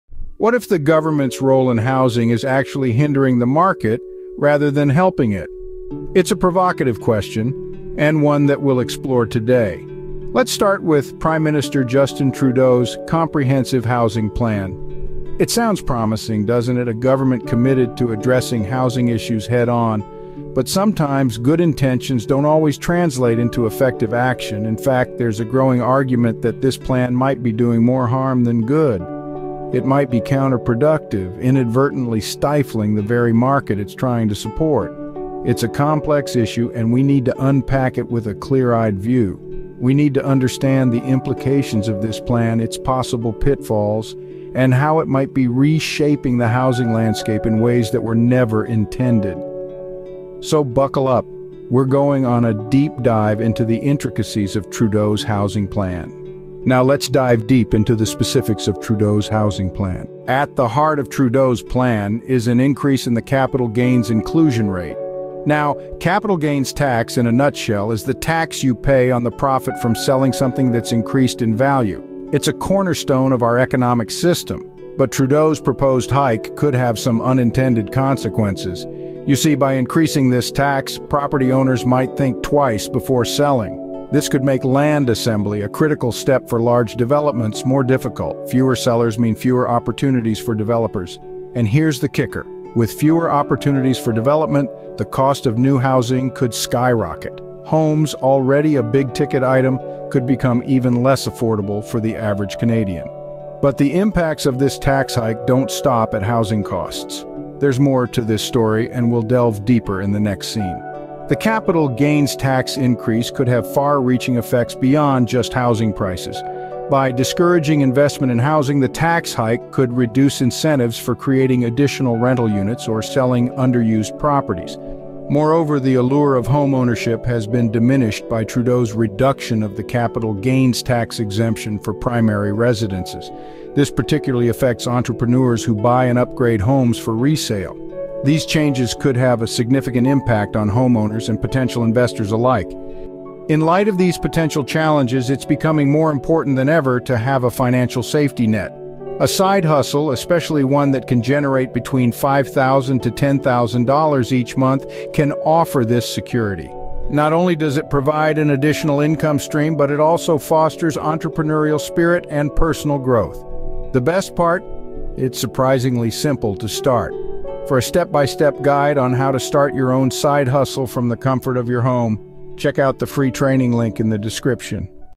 Government's Role in Housing : A Hindrance?
Discover the intricate relationship between government policies and the housing market in Canada in our latest video! We delve into how legislative changes can affect housing prices, availability, and overall market stability. Additionally, we explore why side hustles are becoming increasingly vital for Canadians seeking financial security and independence. Whether you're a homeowner, renter, or aspiring entrepreneur, this video provides valuable insights to navigate Canada's evolving economic landscape.